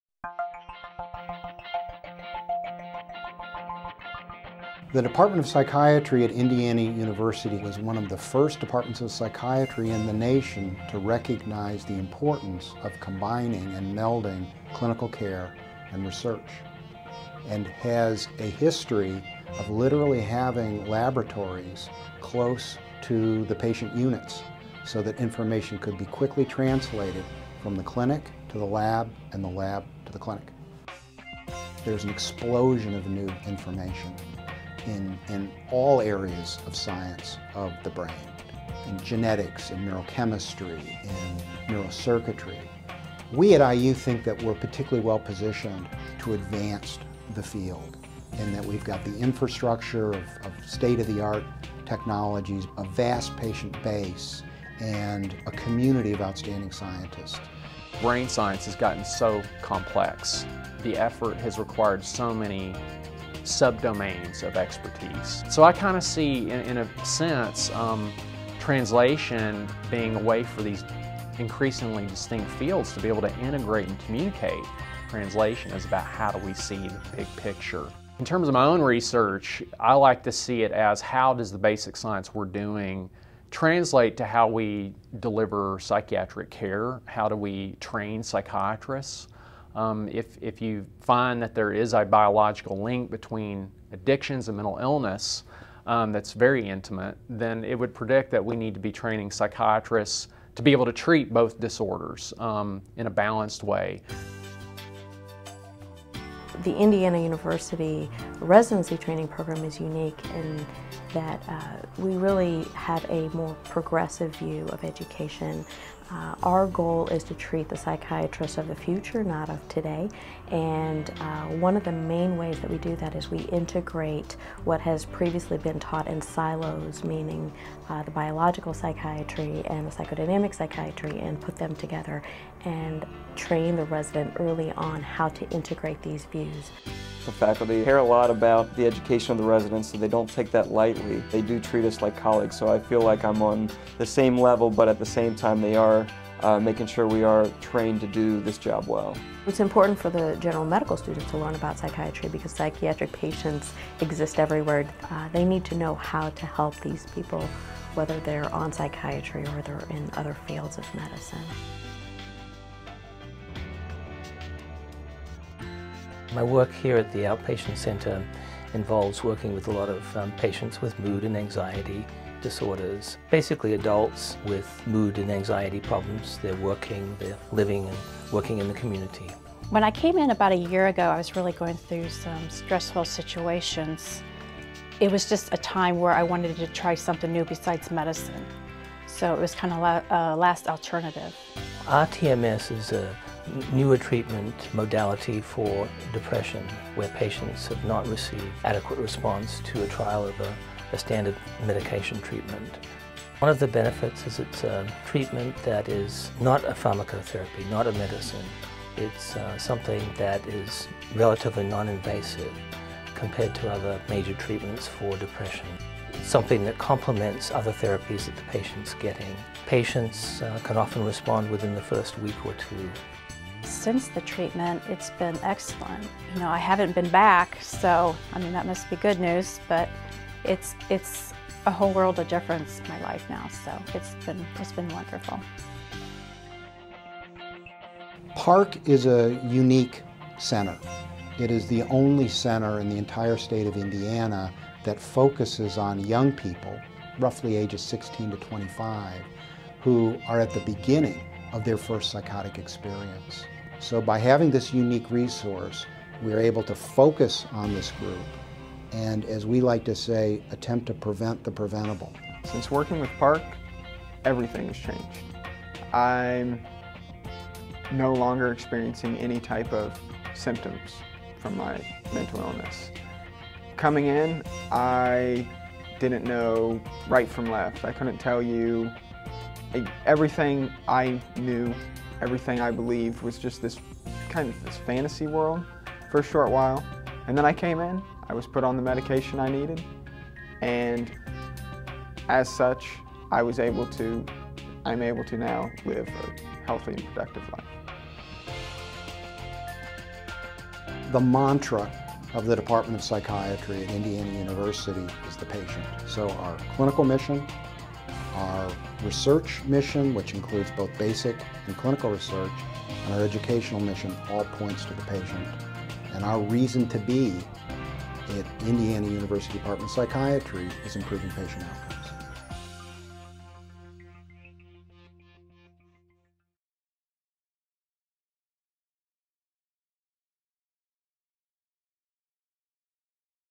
Growing For the Future, Department of Psychiatry at Indiana University School of Medicine
As a specialty department within the second largest allopathic medical school in the country, the Department of Psychiatry at Indiana University School of Medicine provides exceptional education for medical students, psychiatric residents and fellows, and clinical psychology interns. Our treatment programs, spread over six distinct clinical facilities, serve patients ranging from pre-schoolers through older adults.